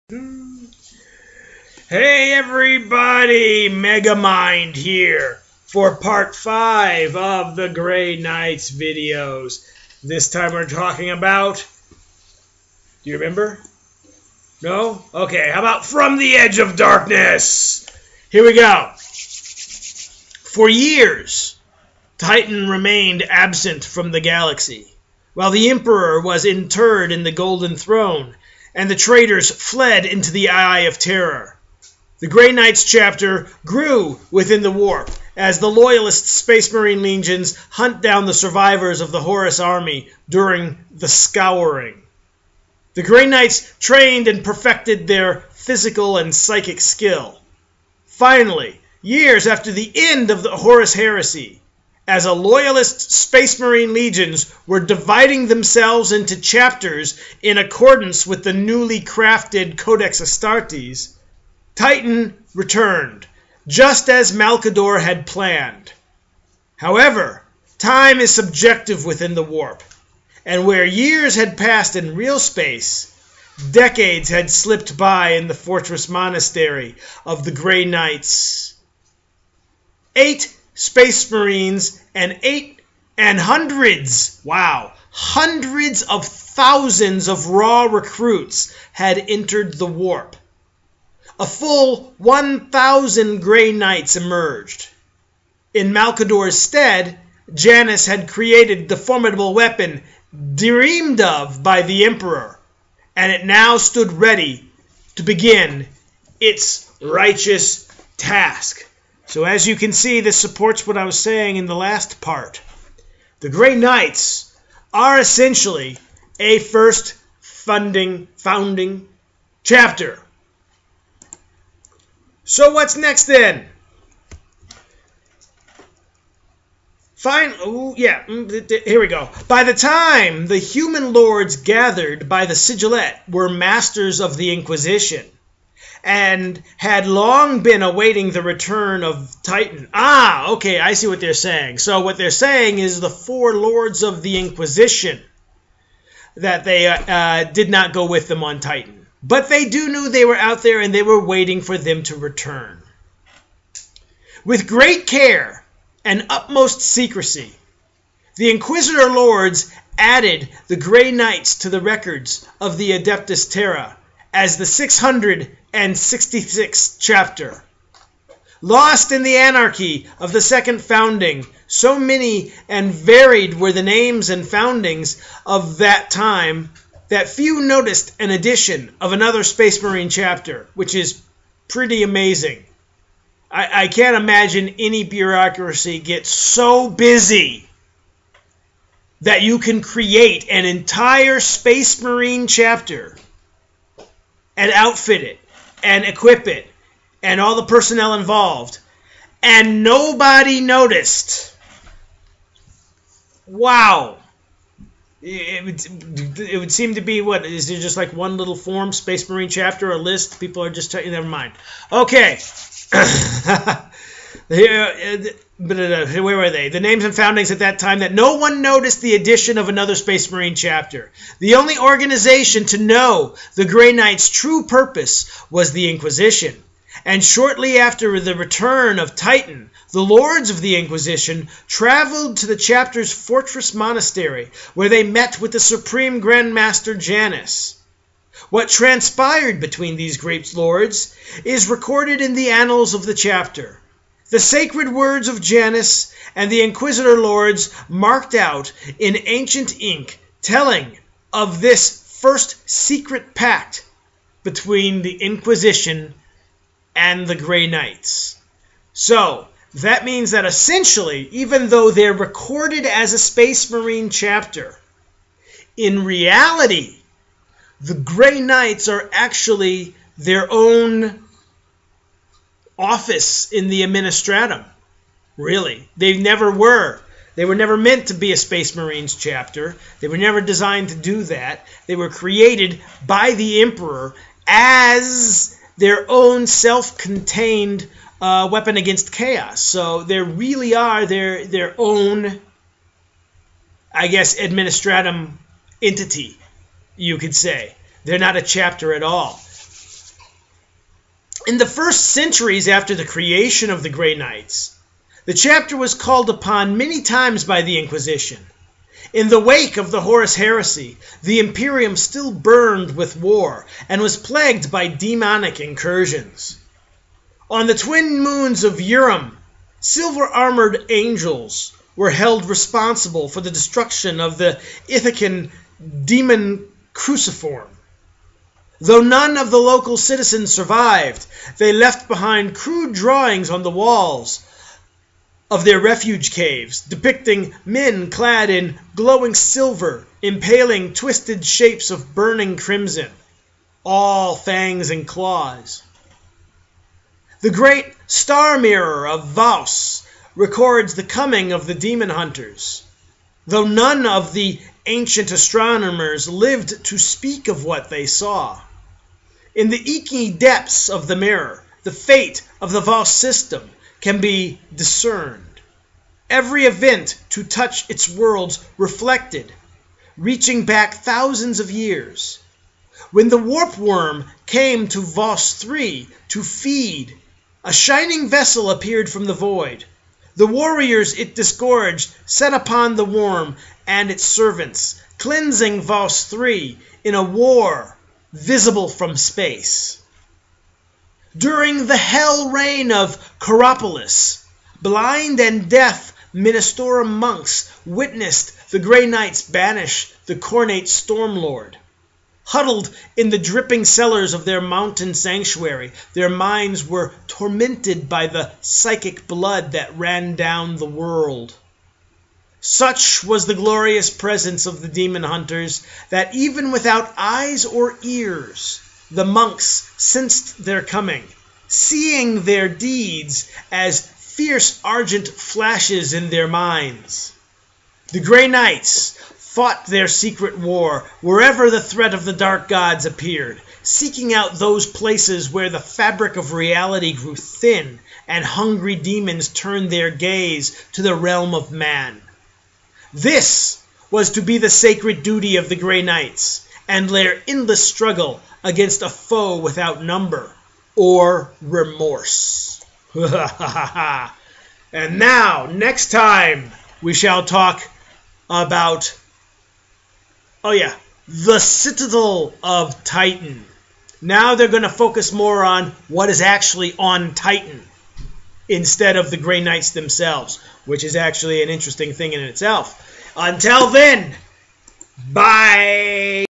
GREY KNIGHTS FLUFF PT 5:From the Edge of Darkness, warhammer 40k
Grey Knights part 5: From the Edge of Darkness. They must have written this Codex after watching Star Trek: Into Darkness. Warhammer 40k